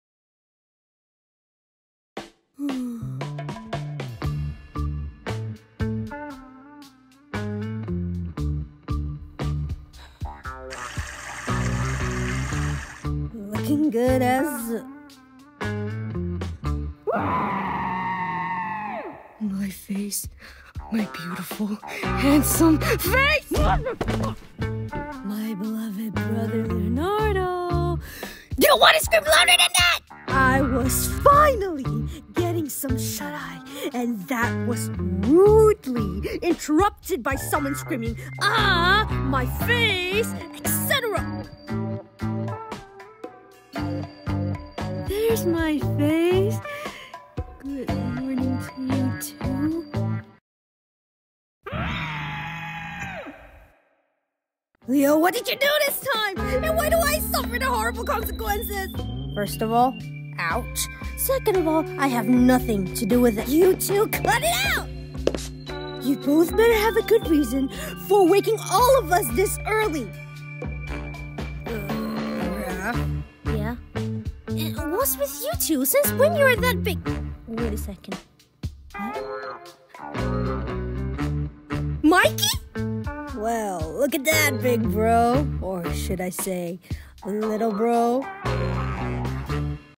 Body Switch (ROTTMNT ANIMATIC FANDUB) ANIMATIC BY Len and Marci
SO THE BROTHERS SWITCHED BODIES FOR A DAY!

I HAD PERMISSION FROM THE ANIMATIC CREATORS TO DUB THIS SO PLS SUPPORT THE ORIGINAL POST ON TWITTER!!